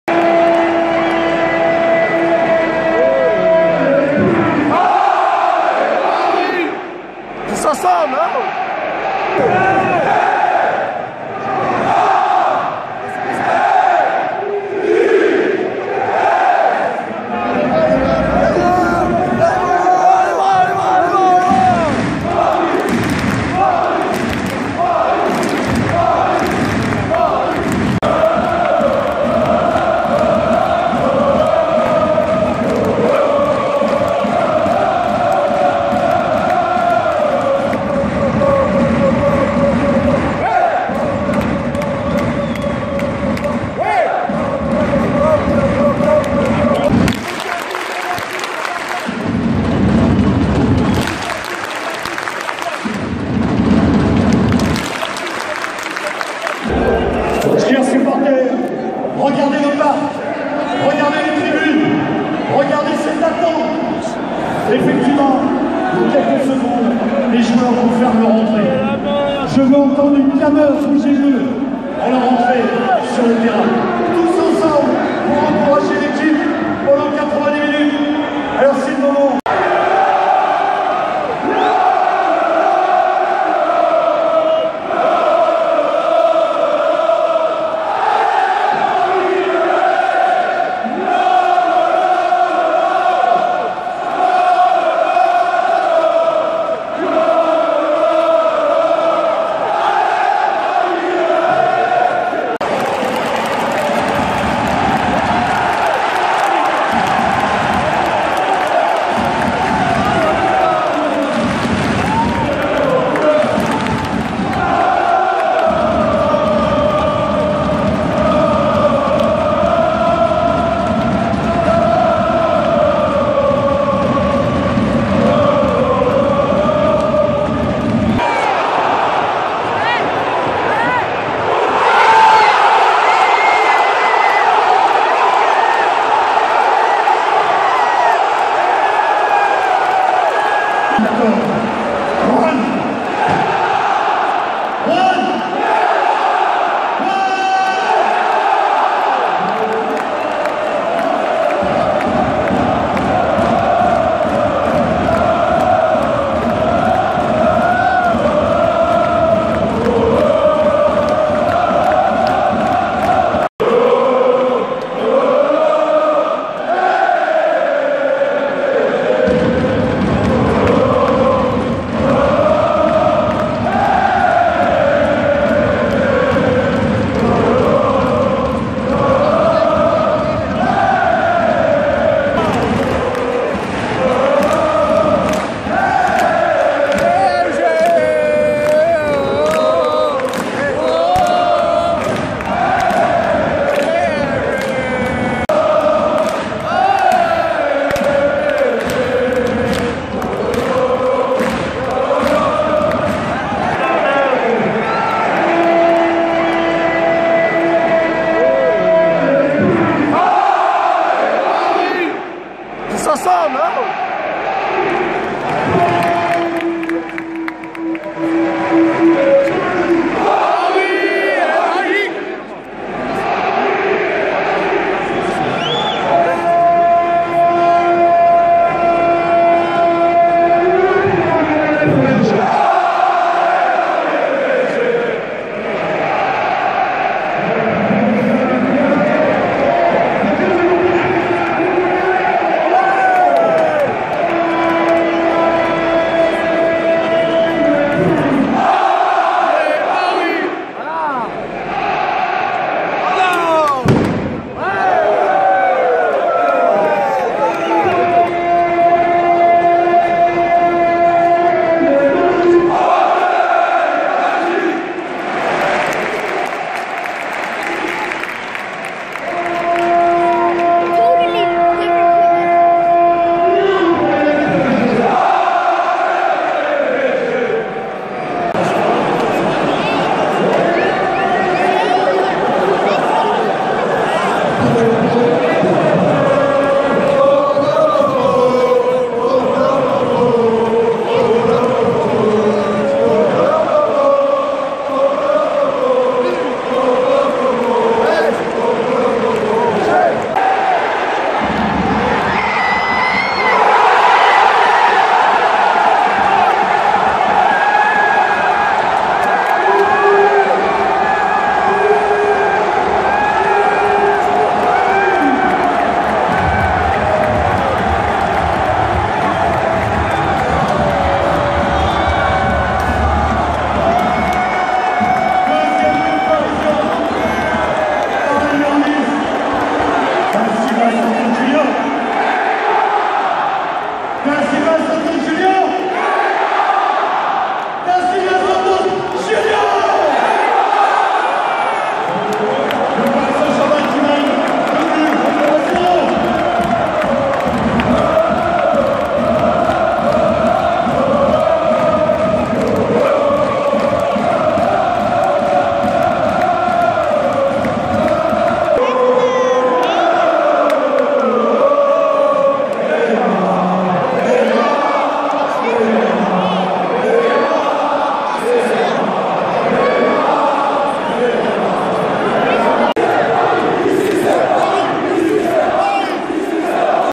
Psg Vs Liverpool 28.11.2018 Ambiance Auteuil 🔥
Petit film sur l'ambiance du côté de Auteuil...